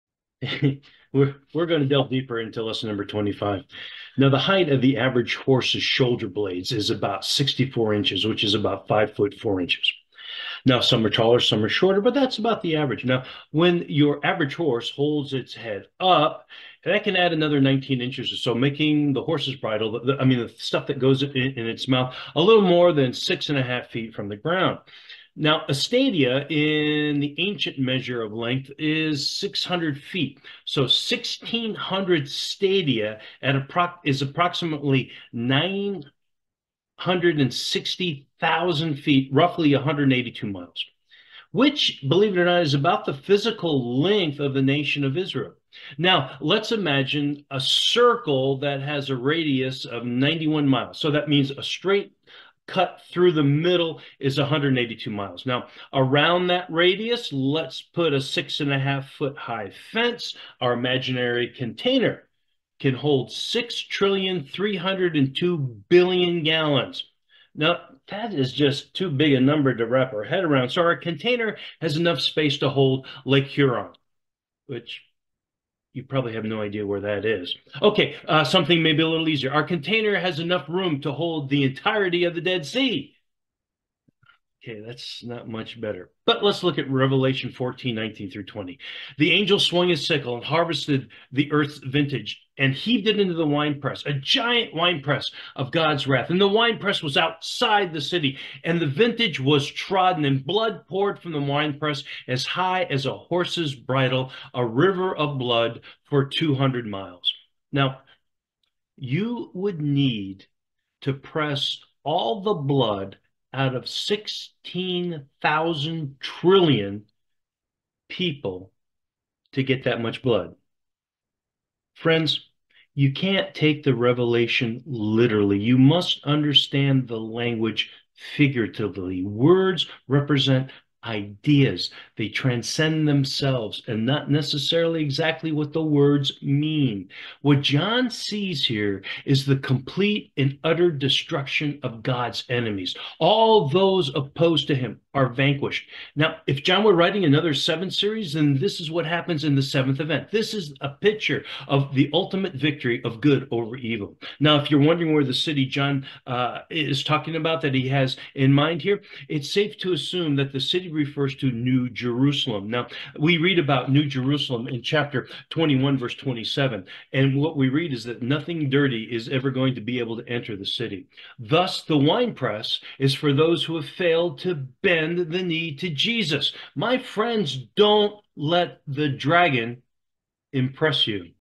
Delve Deeper #25 5 That's A Lot of Blood
Ok, you math wizards time to check my calculations. The amount of blood that comes from the wine press is astronomical.  If you go with a literal interpretation  "The winepress was outside the City. As the vintage was trodden, blood poured from the winepress as high as a horse's bridle, a river of blood for two hundred miles." Rev 14:20 (MSG).  That's a lot of blood.